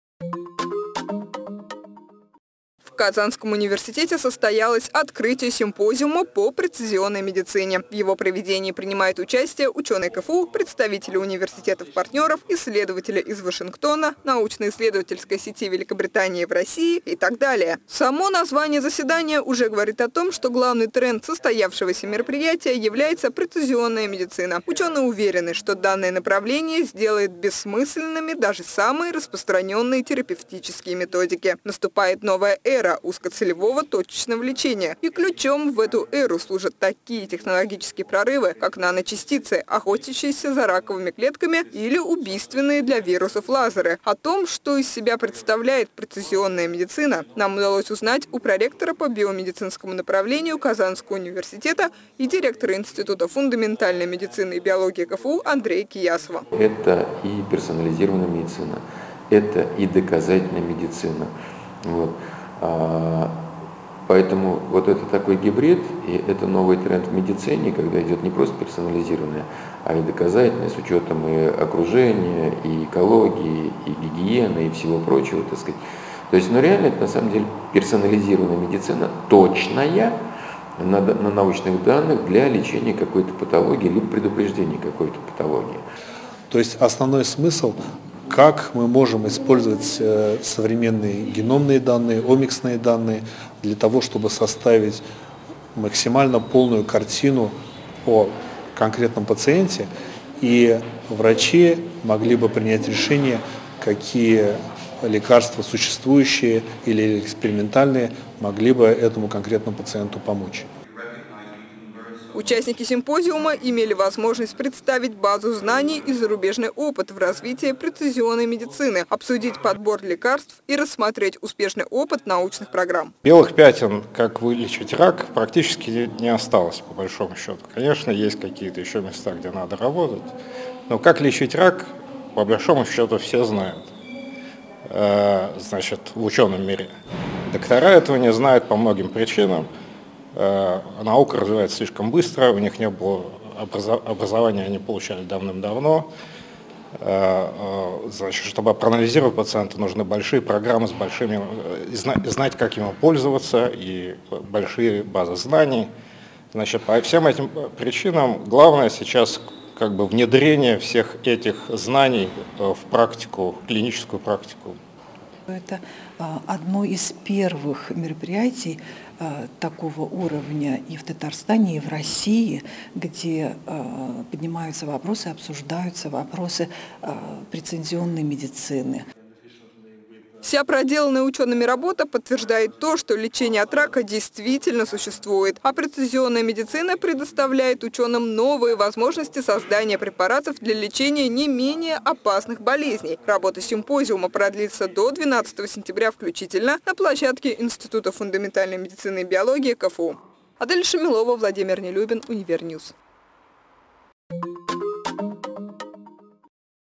Учёные научились лечить болезнь до её появления
Медицинская помощь, основанная на лечении пациентов уже после того, как они заболели, стала вчерашним днём. Уже сегодня мир имеет возможность начинать лечение гораздо раньше – потенциально, даже до появления первых симптомов заболевания. Появились инструменты для того, чтобы эффективно прерывать болезнь на максимально ранних стадиях и предотвращать её развитие. Об этом пошла речь на состоявшемся в КФУ симпозиуме.